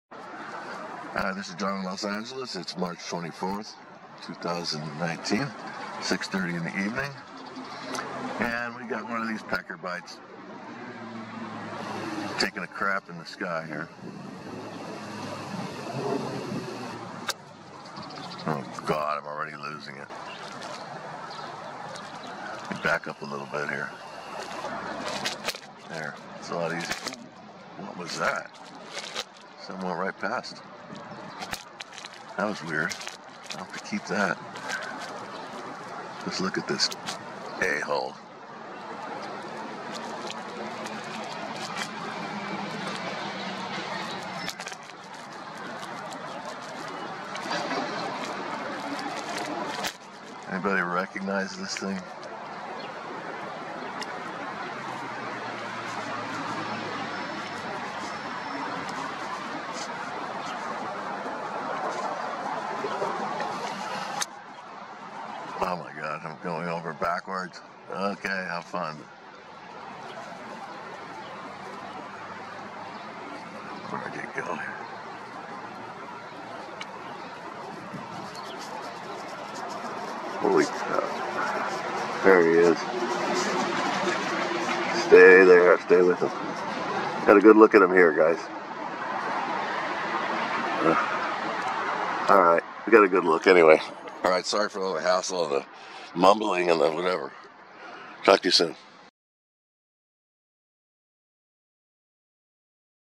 John’s Crossby and UFO
Depth perception deception.   A ufo shows up after crossby.   Amazing capture by a premier photographer and videographer John Graf.  John is a huge anti-chemtrail activist. While filming chem trails, captured this beauty.